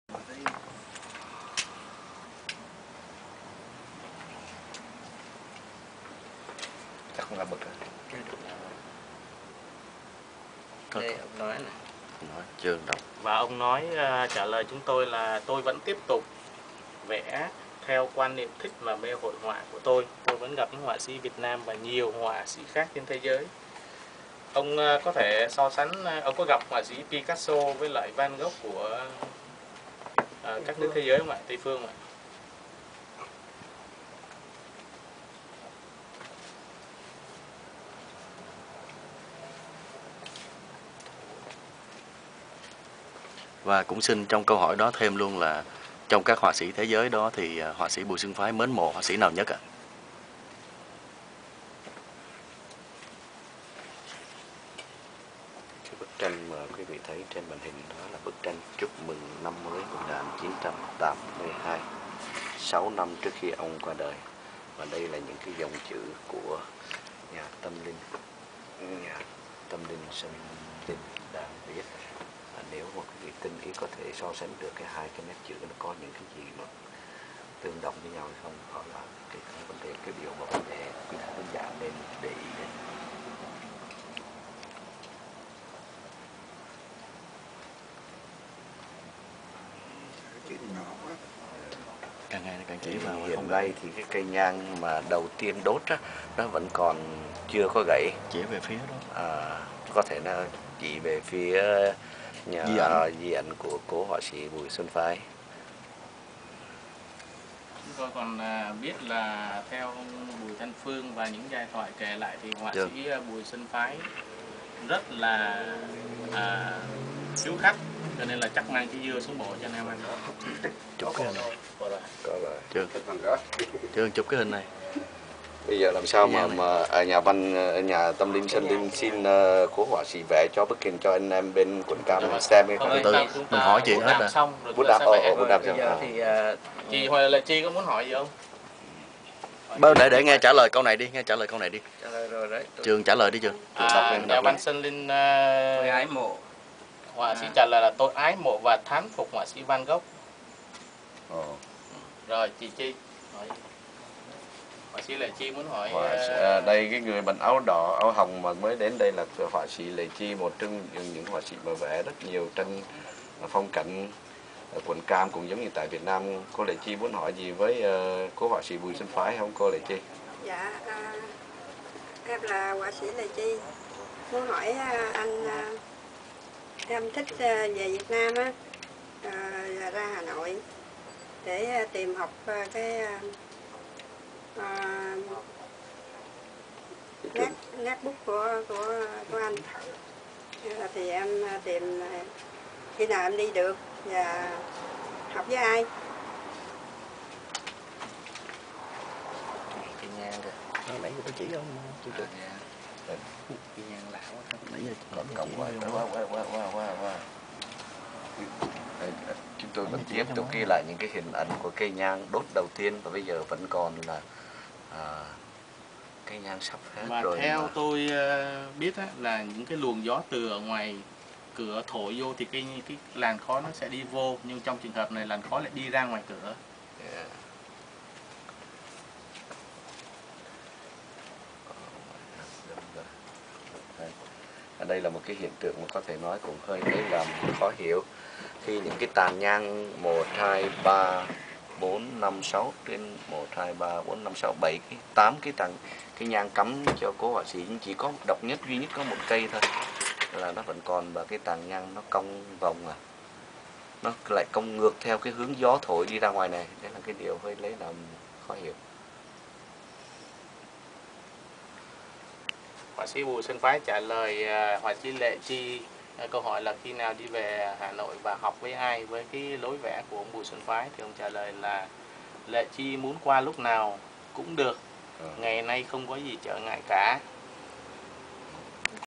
Cầu hồn cố họa sĩ Bùi Xuân Phái lên nói chuyện và vẽ tranh - phần 10
Nhà văn/nhà tâm linh Sơn Linh cầu hồn cố danh họa Bùi Xuân Phái lên nói chuyện và vẽ tranh, với sự tham dự của một số văn nghệ sĩ và nhà báo tại quận Cam, bang California, Hoa Kỳ.

Còn tiếp - Nhớ đón xem!